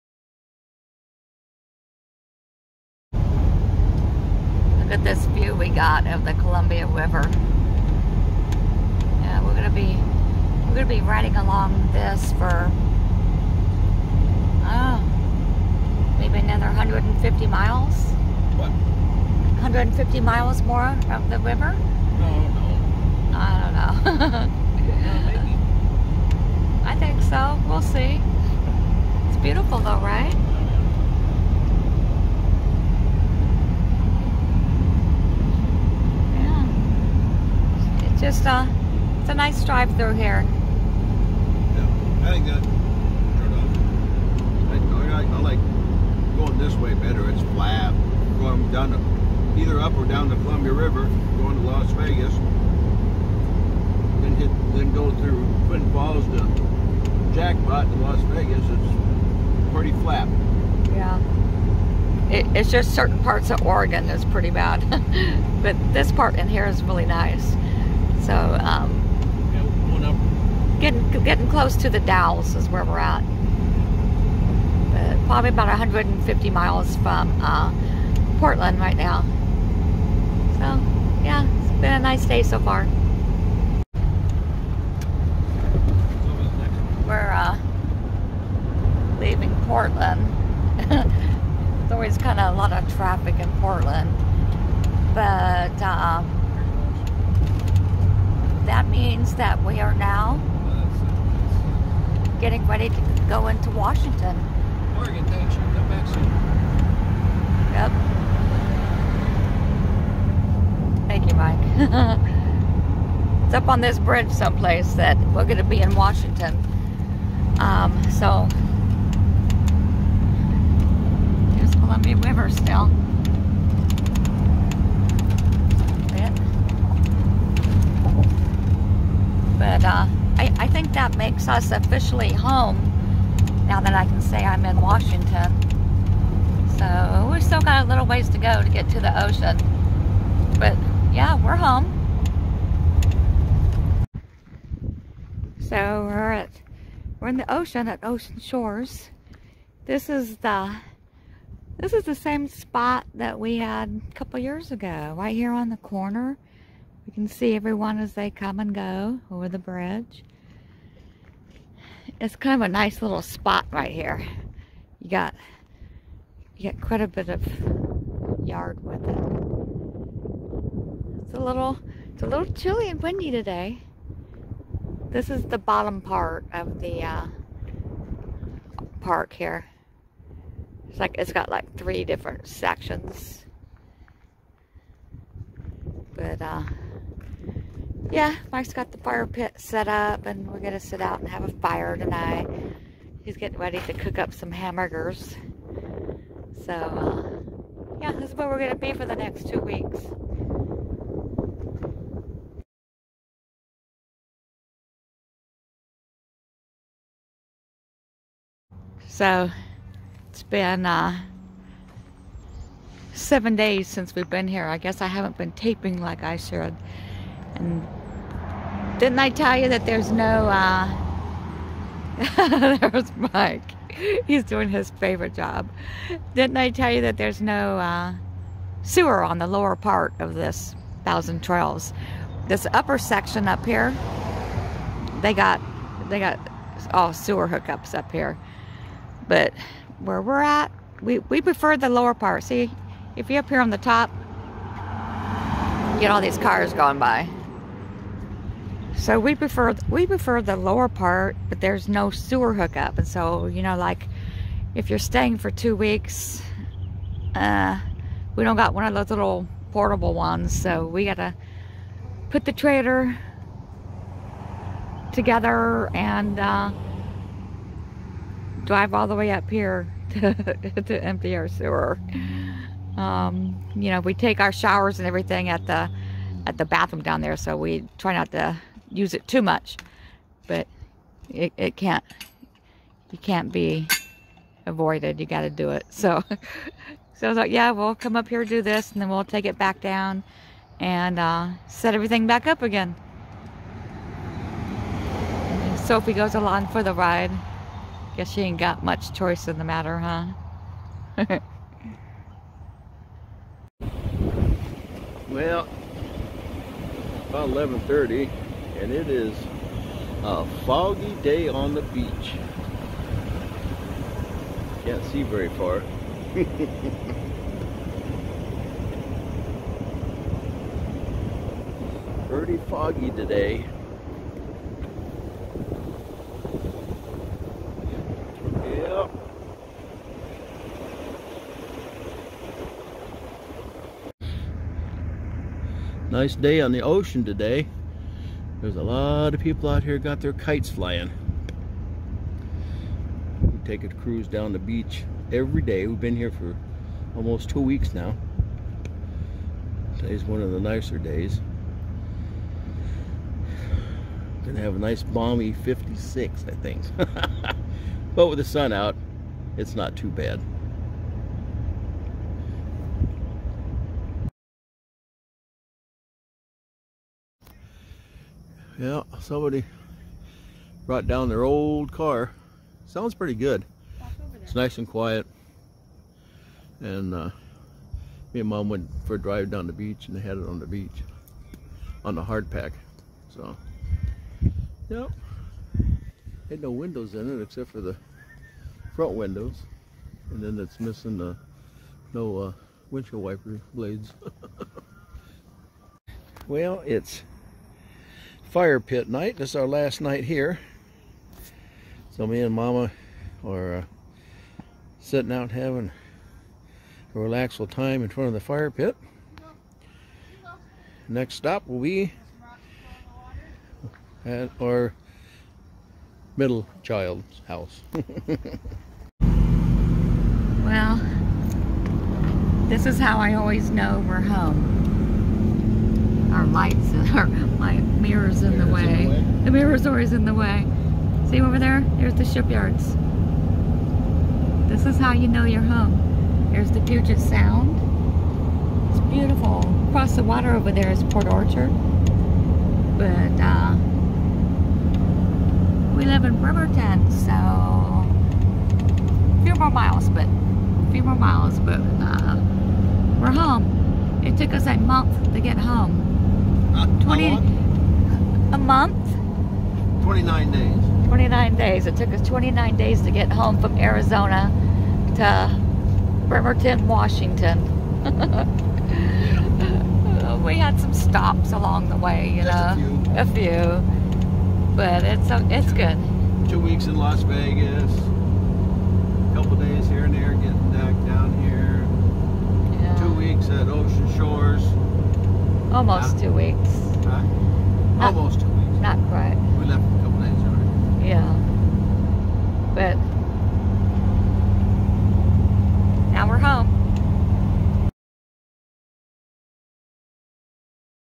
Travel though the Dalles Oregon, Ocean Shores Wa Thousand Trails RV Resort, Beach Driving, RV Life
We arrive in Ocean Shores Washington.. We always go to the beach when we leave Arizona. We take a daily drive on the beach to go to the post office.. Good times